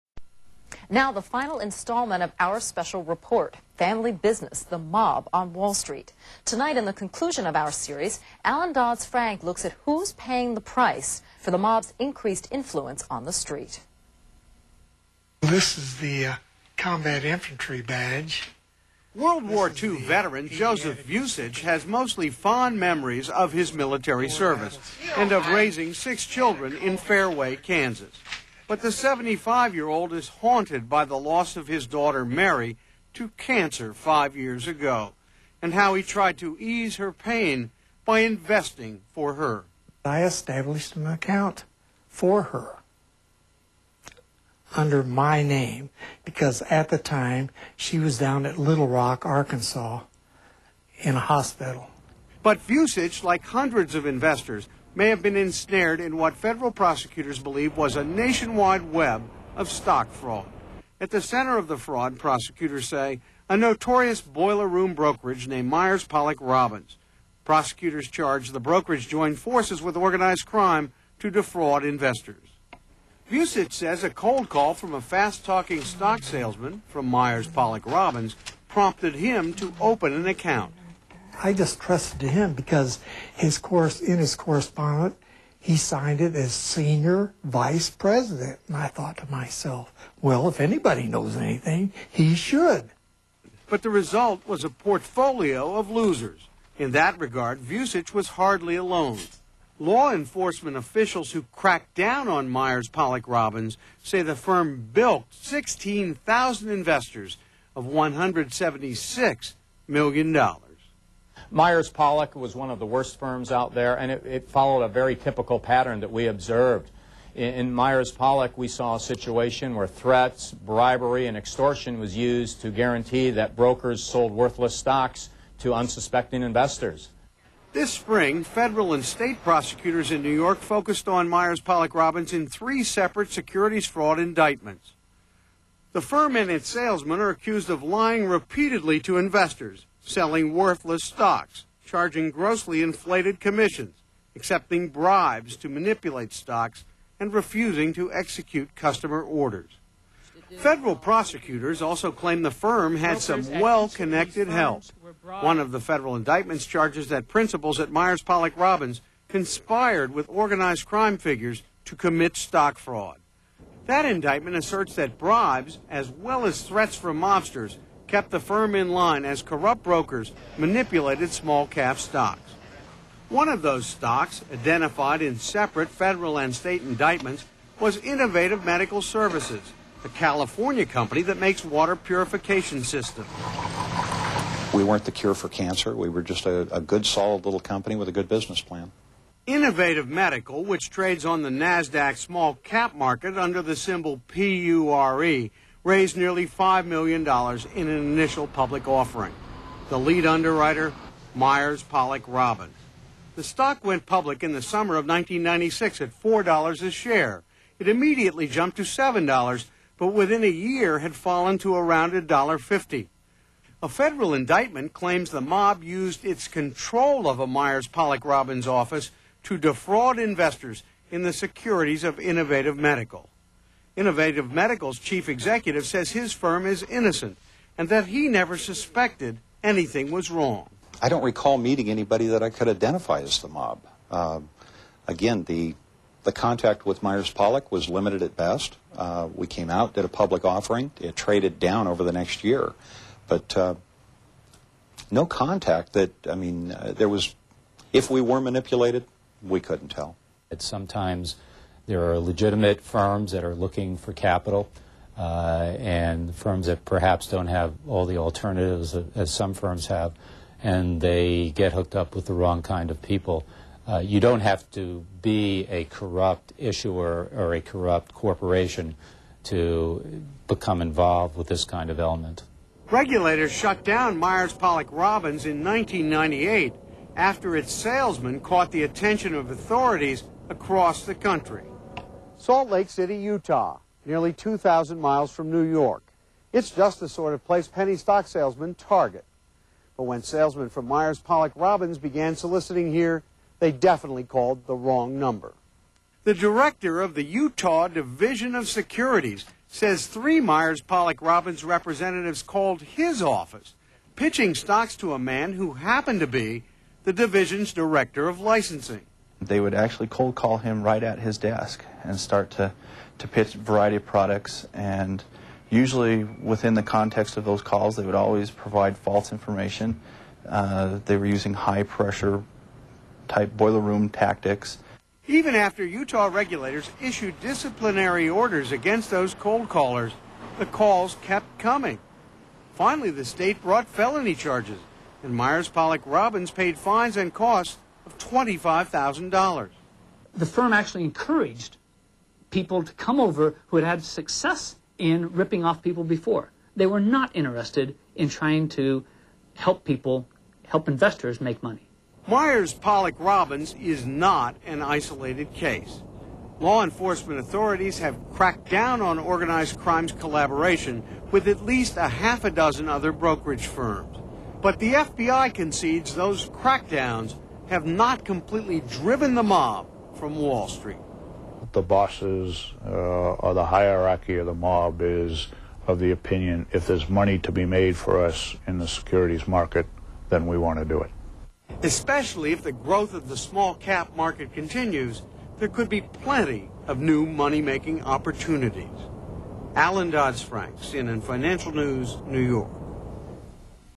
Dad on CNN
2000 CNN story on fraud that featured Dad